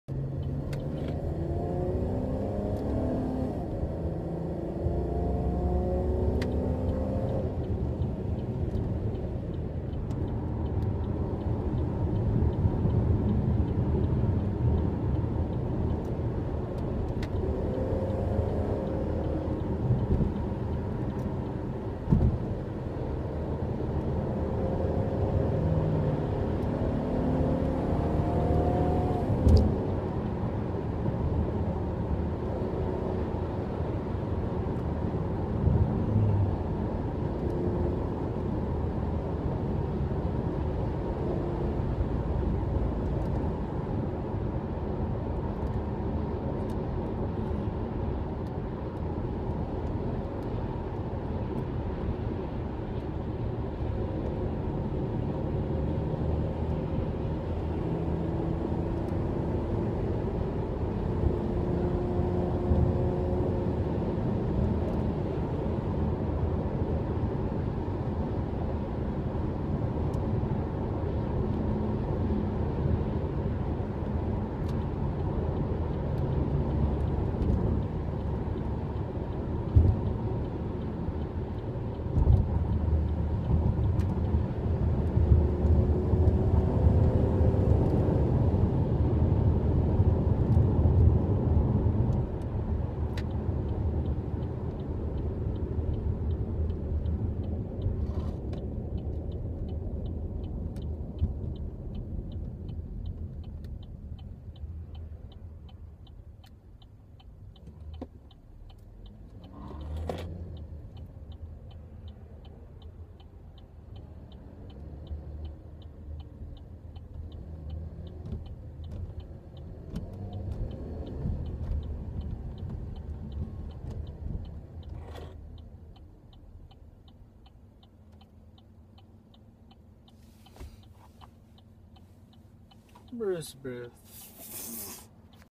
Random freeway drive
random clip of a little freeway drive in the camaro..